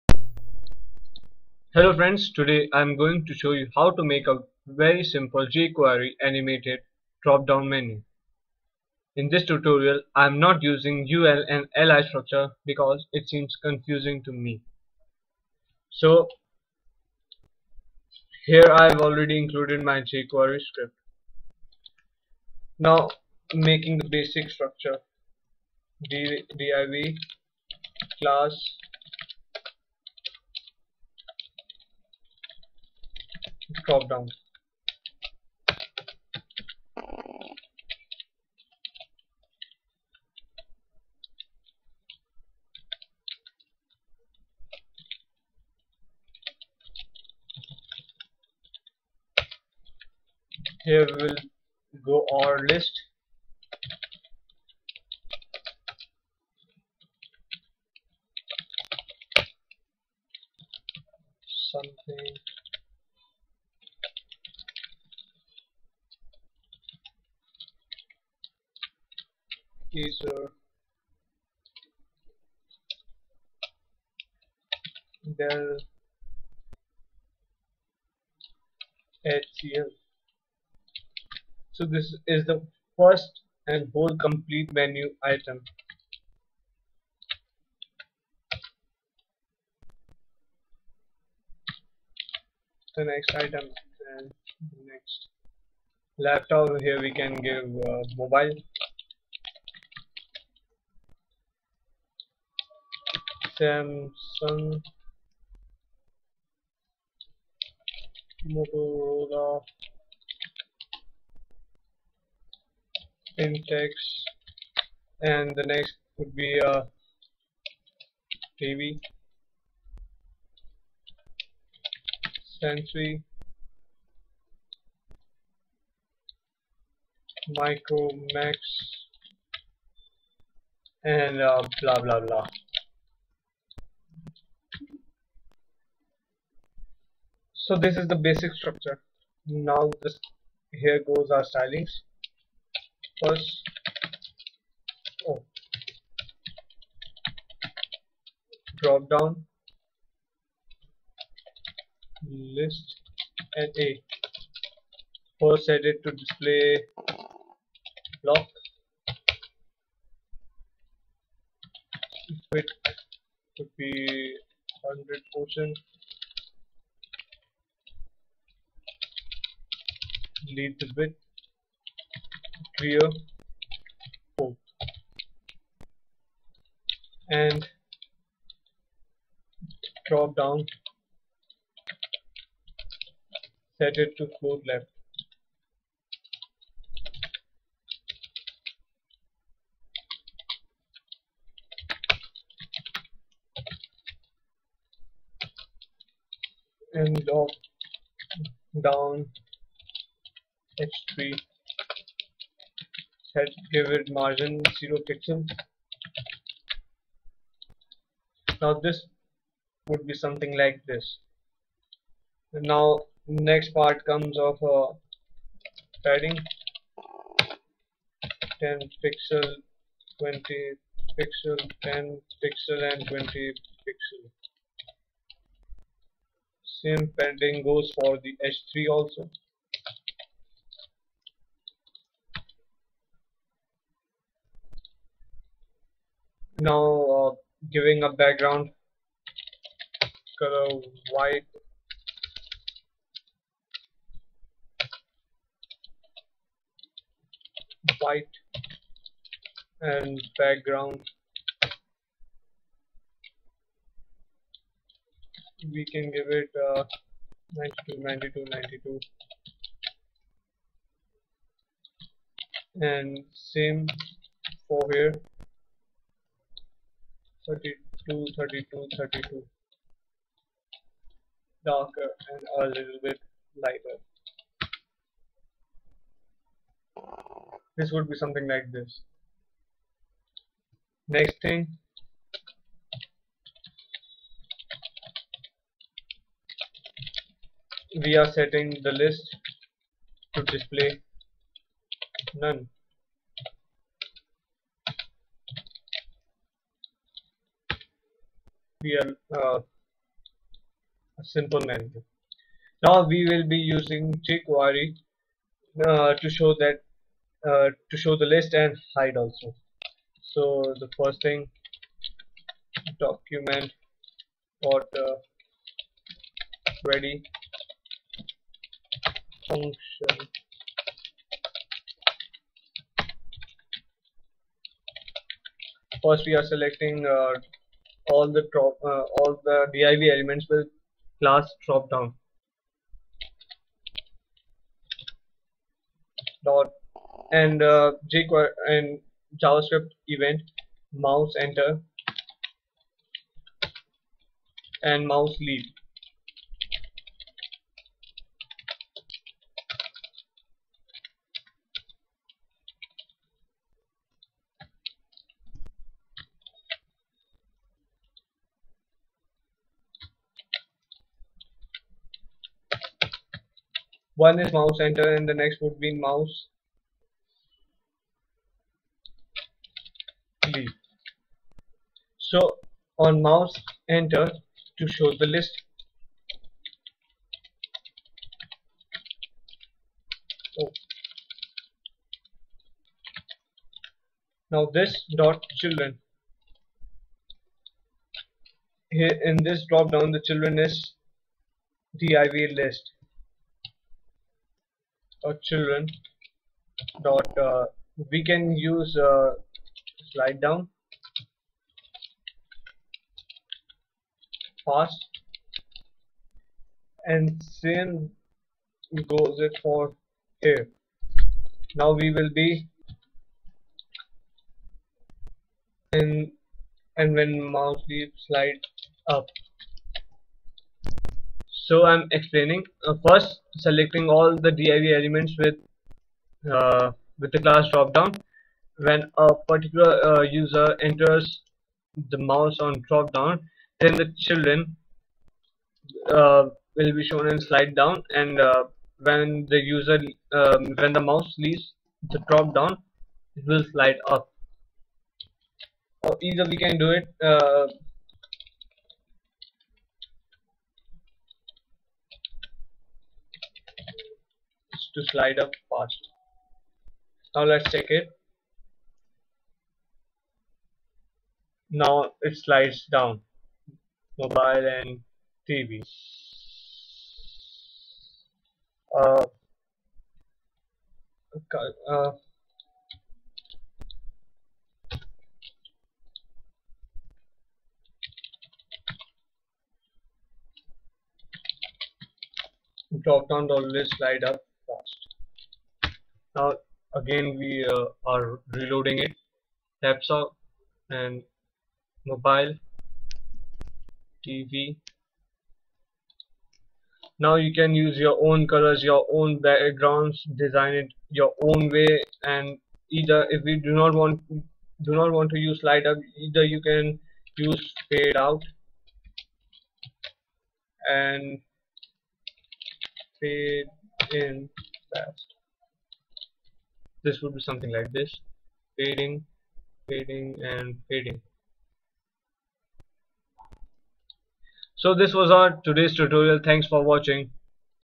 Jquery Animated DropDown Menu
This is Very Simple Jquery Animated Dropdown menu.

.children() -- select the elements present in a div